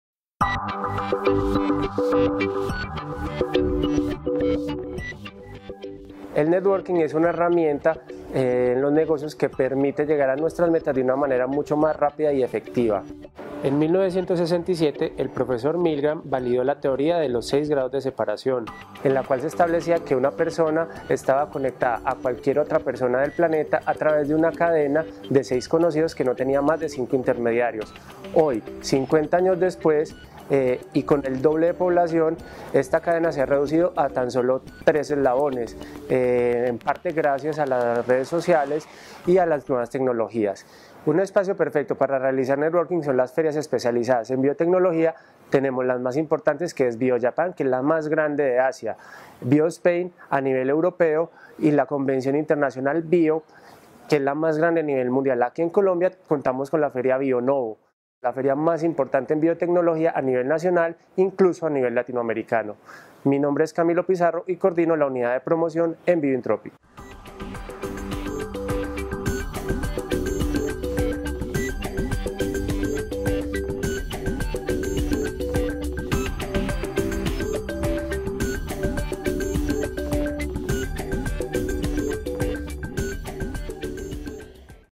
Networking, ferias de Biotecnología y Camilo Pizarro
En este video, Camilo Pizarro, coordinador de la Unidad de Promoción de BIOINTROPIC, nos dice cuáles son las ferias más importantes de Biotecnología en el mundo. Además, Camilo nos comparte un par de datos muy interesantes acerca del networking y la teoría de los seis grados de separación.

Ya estamos preparando Bionovo 2018.

Te compartimos lo que dicen en Wikipedia acerca de la Teoría de los seis grados de separación: Se le llama seis grados de separación a la hipótesis que intenta probar que cualquiera en la Tierra puede estar conectado a cualquier otra persona del planeta a través de una cadena de conocidos que no tiene más de cinco intermediarios (conectando a ambas personas con sólo seis enlaces), algo que se ve representado en la popular frase «el mundo es un pañuelo». La teoría fue inicialmente propuesta en 1930 por el escritor húngaro Frigyes Karinthy en un cuento llamado Chains.

El concepto está basado en la idea de que el número de conocidos crece exponencialmente con el número de enlaces en la cadena, y sólo un pequeño número de enlaces son necesarios para que el conjunto de conocidos se convierta en la población humana entera.

Recogida también en el libro Six Degrees: The Science of a Connected Age del sociólogo Duncan Watts, y que asegura que es posible acceder a cualquier persona del planeta en tan sólo seis «saltos».

Recuerda que puedes encontrar más acerca de la aceleradora de negocios de Biotecnología, BIOINTROPIC,